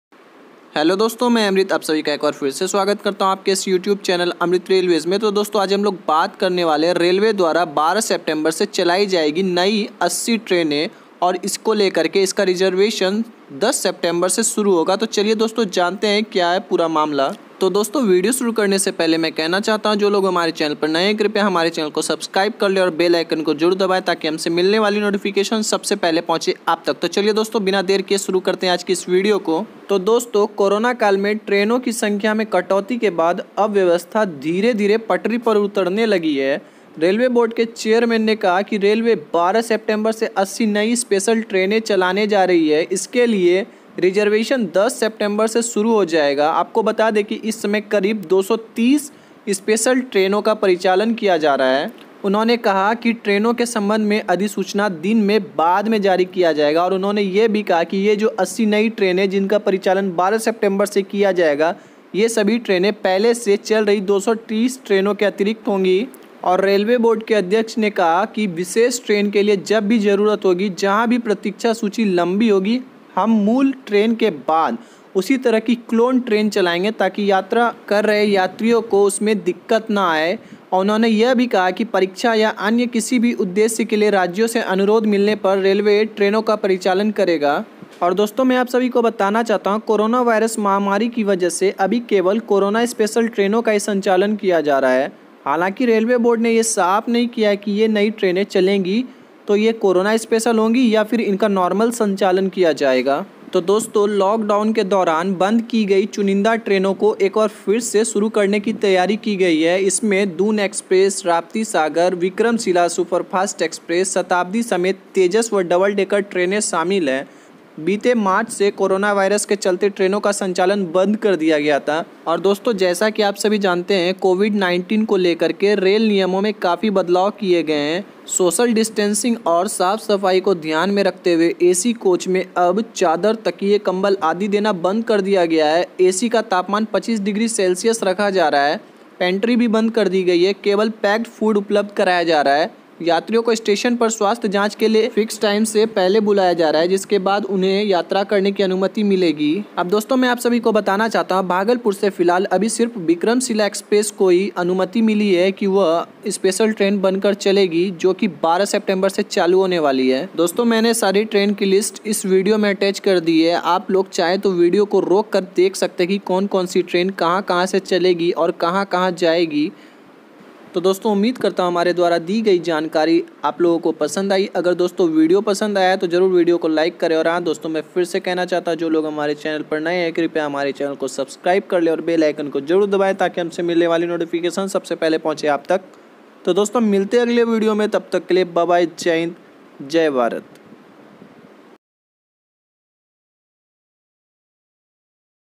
भागलपुर की विक्रमशिला एक्सप्रेस समेत देश भर में 80 ट्रेनों का किया जाएगा परिचालन || Vikramshila Exp
Indian Railways will operate 40 pairs of new special trains from September 12, reservation for which will begin from September 10, Railway Board Chairman VK Yadav said on Saturday (September 5, 2020).

"Eighty new special trains or 40 pair of trains will start operations from September 12. Reservations will begin from September 10. This will run in addition to the 230 trains already in operation," he said.

These new trains will run in addition to 230 trains that are already in operation, the Railway Board Chairman added.

He also said that the Railways will run clone trains for waiting-list passengers

"Wherever there is a demand for a particular train, wherever the waiting list is long, we will run a clone train ahead of the actual train, so that passengers can travel," the railway board chairman said.

Yadav also said the Railways will run trains whenever there is demand from states for exams or other similar purposes.